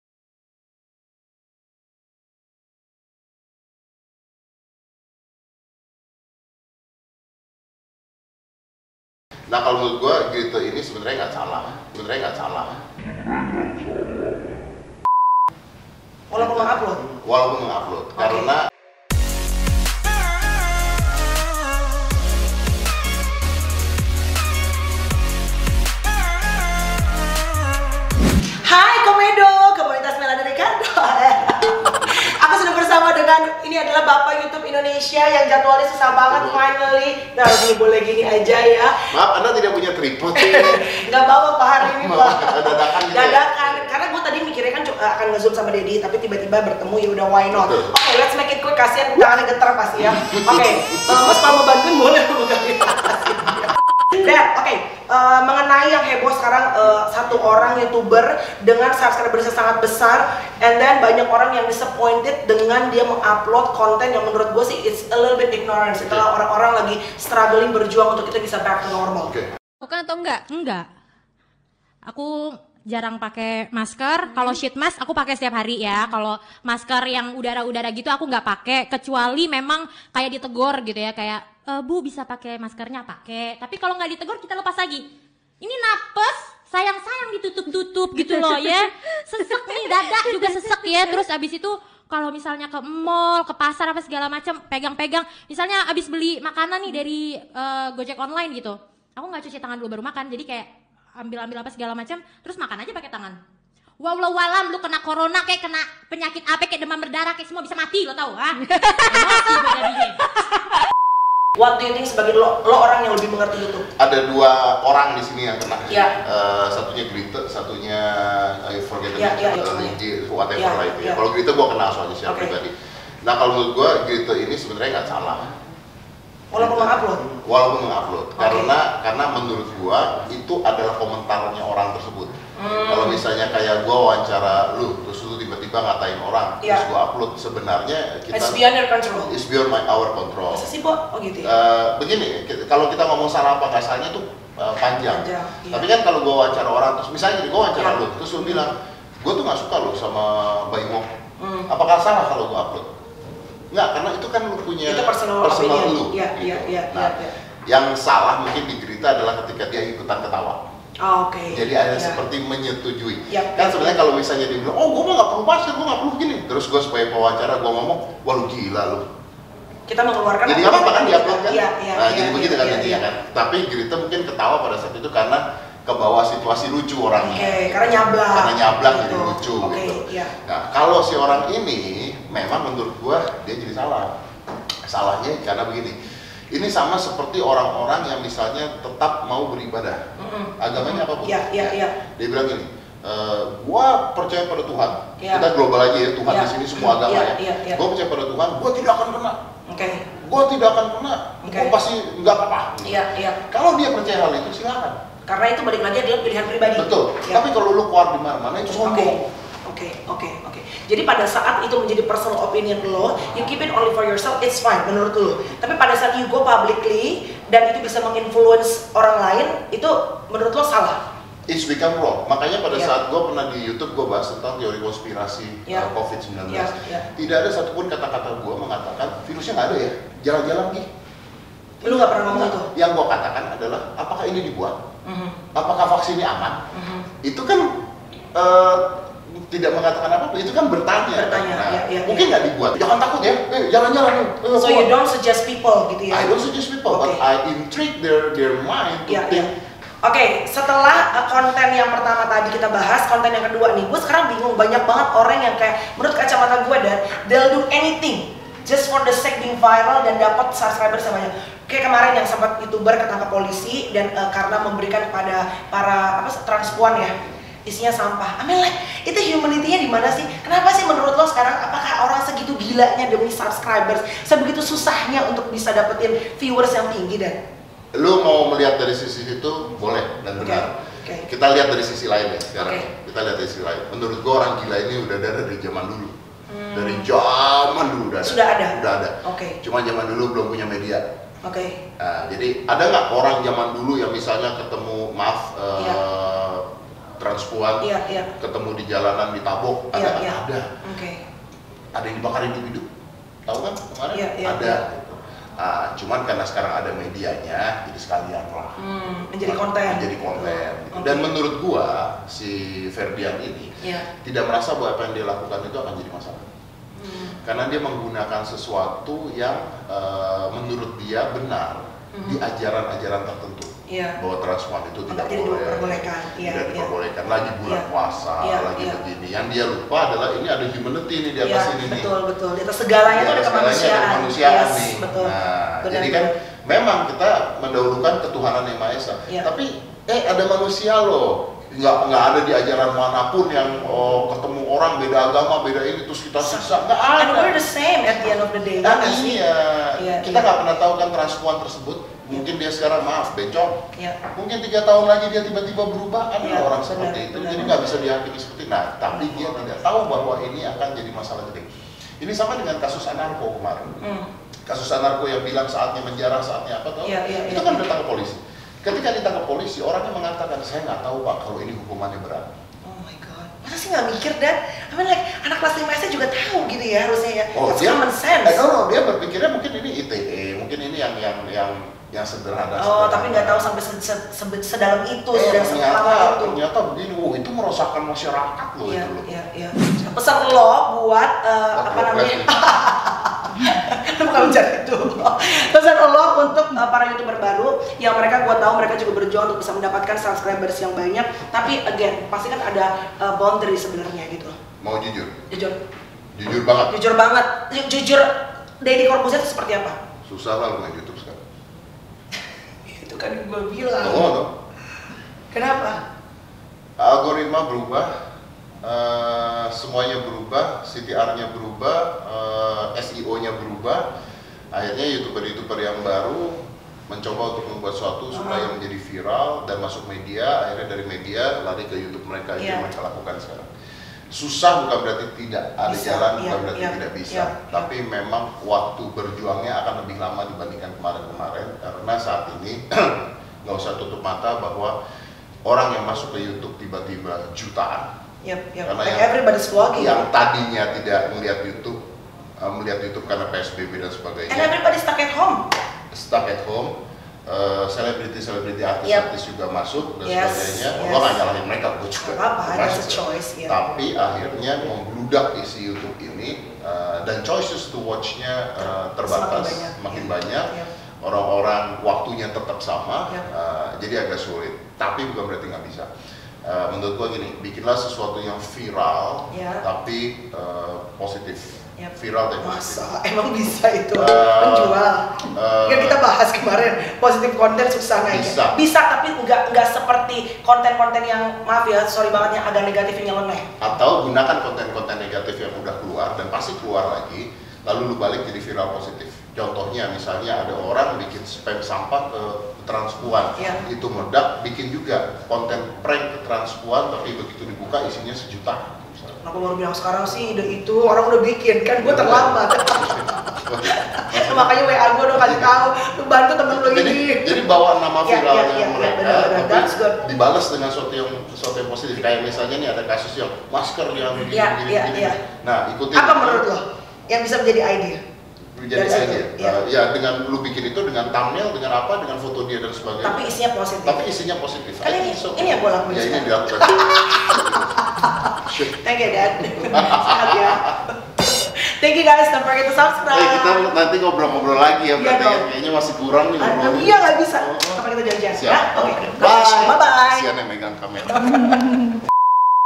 Deddy Corbuzier MEMBUAT STATEMENT MENCENGANGKAN "Menurut gue gritte gak salah!" 123 JAWAB SEMUANYA
Hai guys... welcome back in my youtube channel, kali ini aku mau ngajakin ngobrol DEDDY CORBUZIER lagi nih, kalian penasaran kan!?? selengkapnya di tonton sampai habis ya guys!